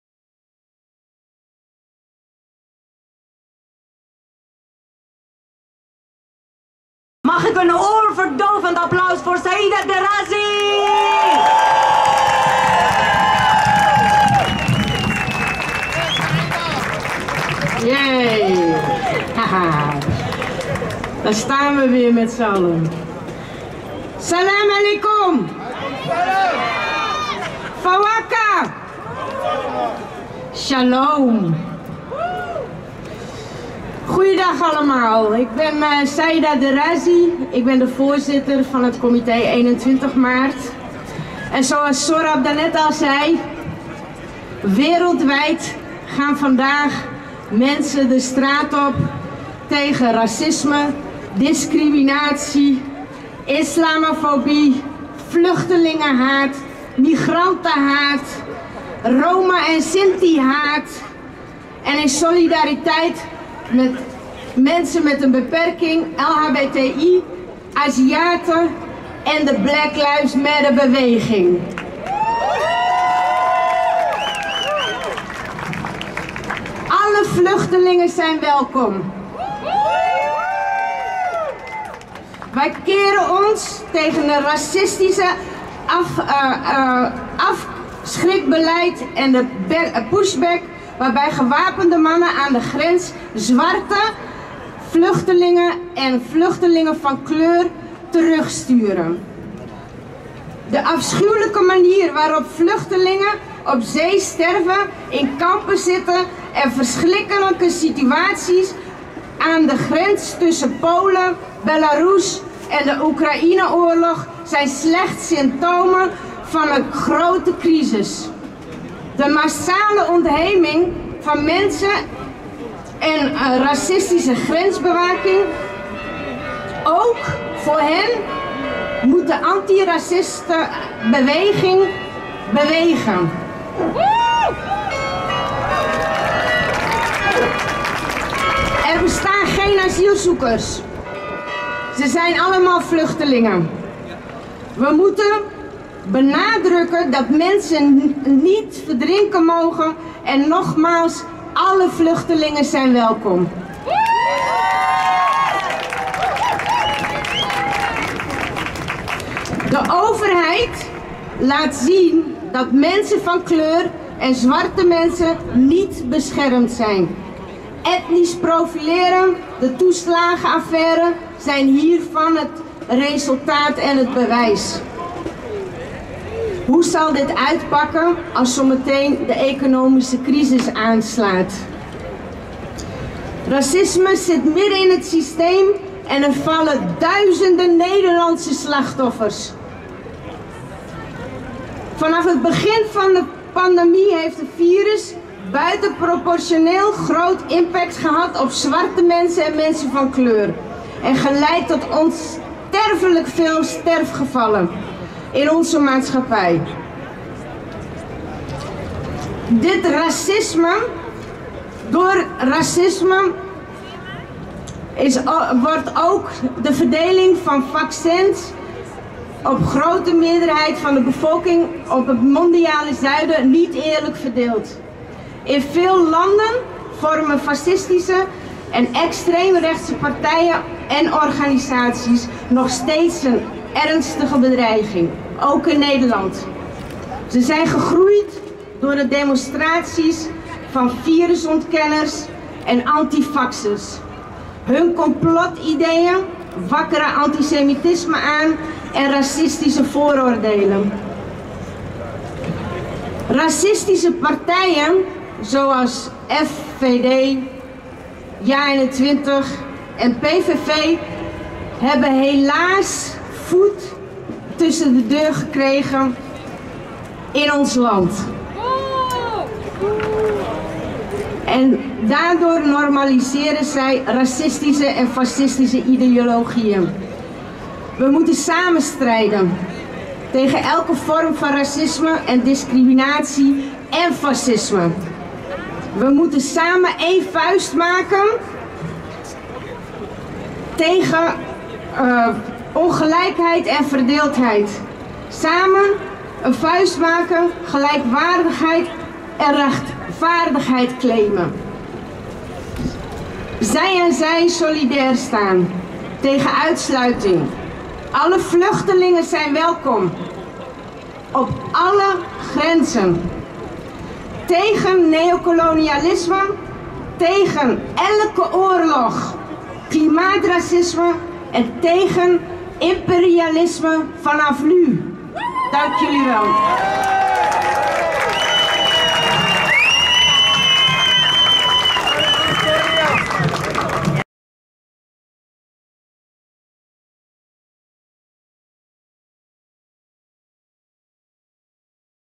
Comité 21 Maart - Saida Derrazi - Demo Dam-Dokwerker Amsterdam
Demonstratie SAMEN TEGEN RACISME EN DISCRIMINATIE 2022
19 maart 2022
AMSTERDAM - VAN DE DAM NAAR DE DOKWERKER

Saida Derrazi, Activist/Public Affairs, Strategic advisor, Coördinator, Coach, movements and collectief builder, in Human, Women and Children Rights.